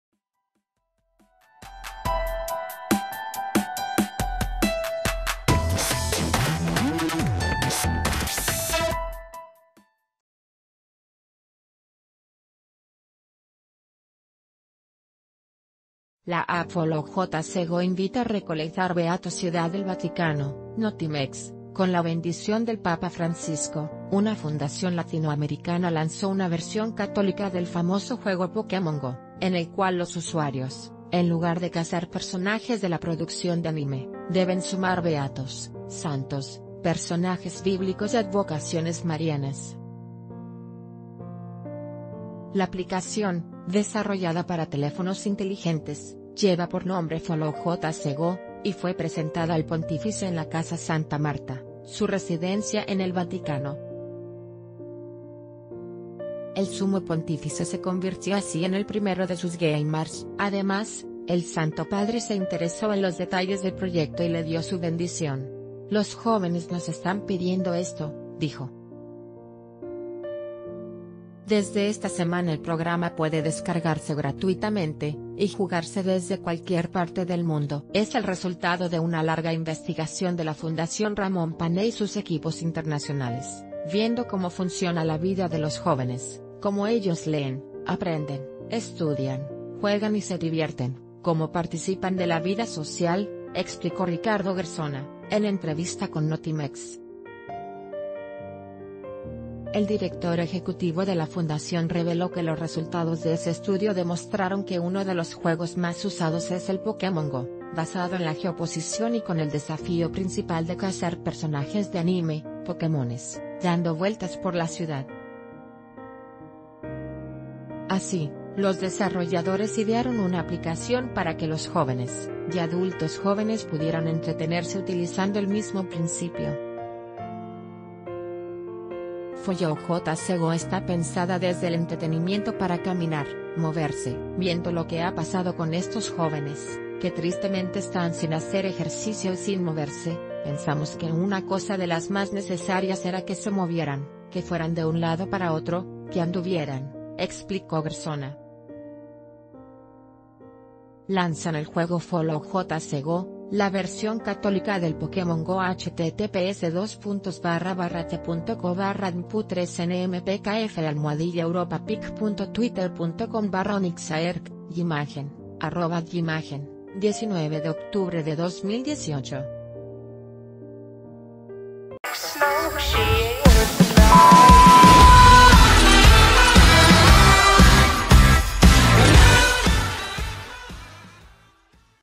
Los santos sustituirán a los famosos pokemones
La app “Follow JC Go” invita a recolectar beatos CIUDAD DEL VATICANO (Notimex).— Con la bendición del papa Francisco, una fundación latinoamericana lanzó una versión católica del famoso juego “Pokémon Go”, en el cual los usuarios, en lugar de cazar personajes de la producción de anime, deben sumar b...